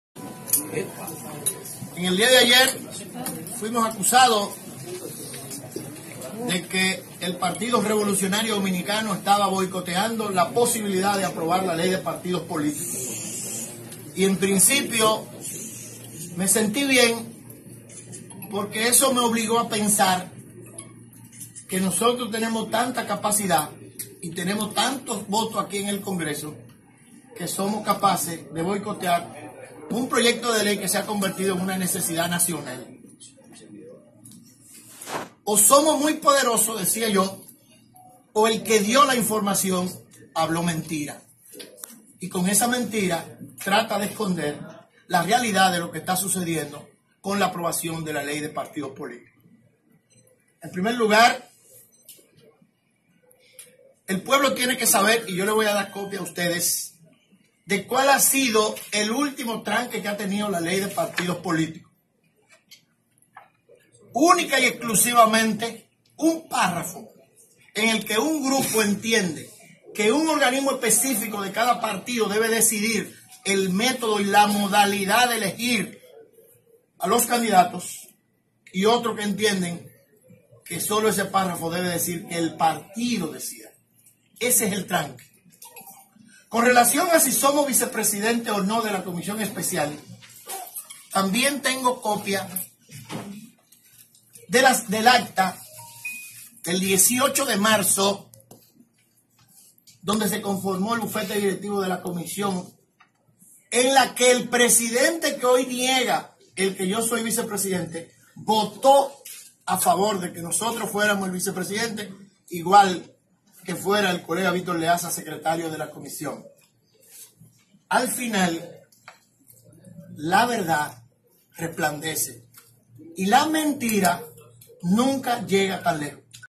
Radhamés Gonzalez acusó al presidente de la Camara de Diputados de darle largas al tema de la ley de
Por Darwin Feliz Matos

Santo Domingo.- El vocero de los diputados del Partido Revolucionario Dominicano (PRD), Radhamés González, acusó al presidente de la Cámara de Diputados, Rubén Maldonado de darle largas al tema de la Ley de Partidos, para negociar en el Comité Político del Partido de la Liberación Dominicana (PLD), posiciones en las cámaras legislativas.

González deploró que en el día de ayer haya sido acusada a la bancada del PRD de querer impedir que se apruebe esa importante pieza legislativa la que según dijo se ha convertido en una necesidad nacional.